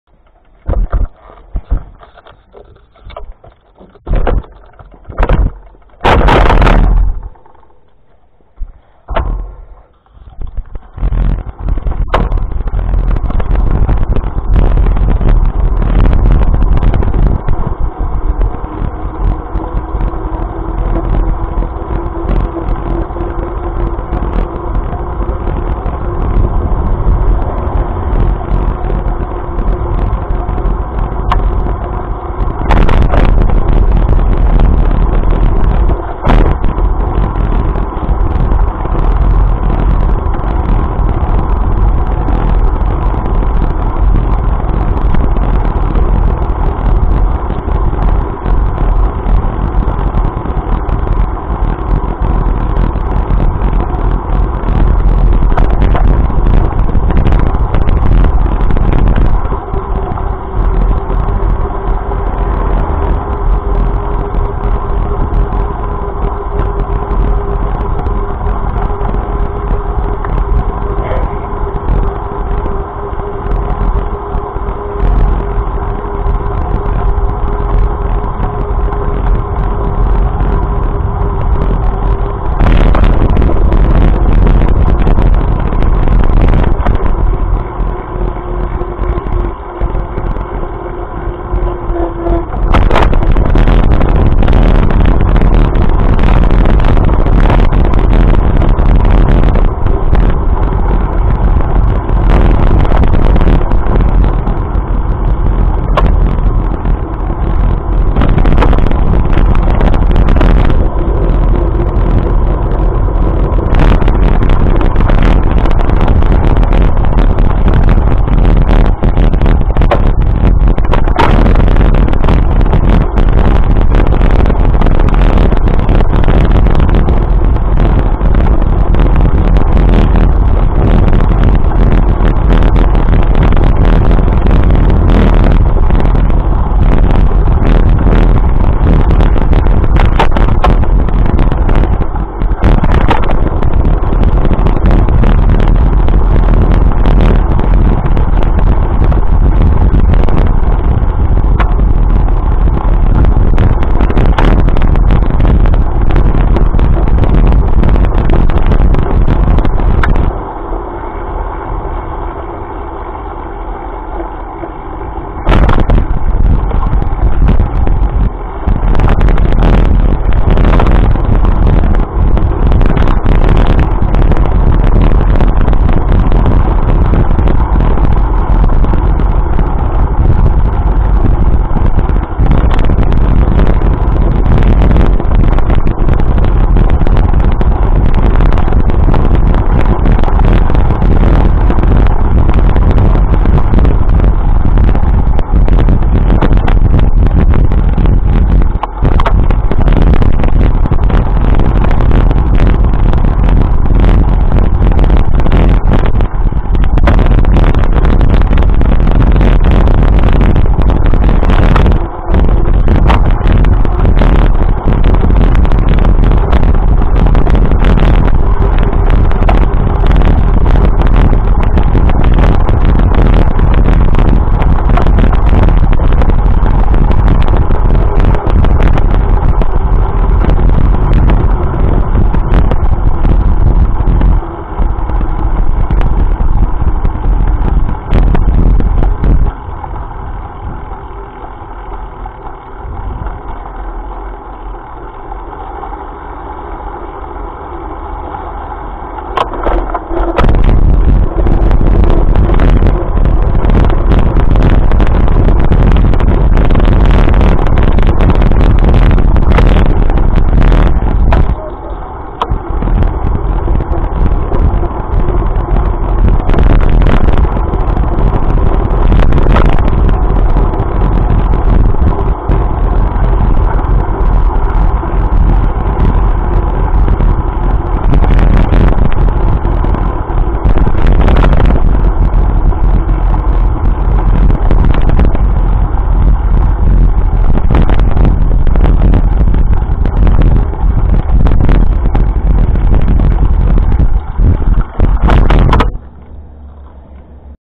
Stukje fietsen door Bottendaal
Stukje fietsen door Bottendaal in Nijmegen met mijn nieuwe camera ;-)